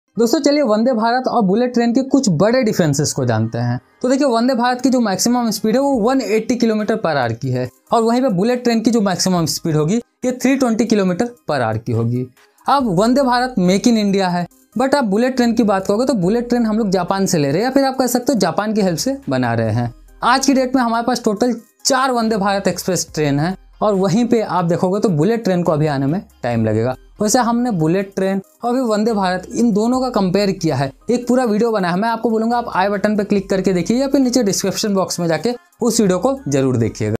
VANDE BHARAT या Bullet Train?
Dear Dosto, Es video mein humlog Vande Bharat aur Bullet Train ki baat karenge.

Please watch the video till end, In case anyone have any queries or suggestions please write it into the comment box.

TechTakaTak-ByHarshvardhan

Jai Hind.